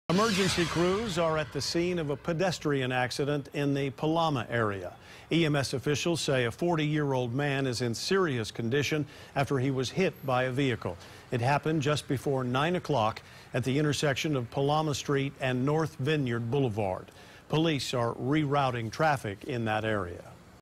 Police investigate Vineyard pedestrian accident
EMS officials say a 40-year-old man is in serious condition, after he was hit by a vehicle.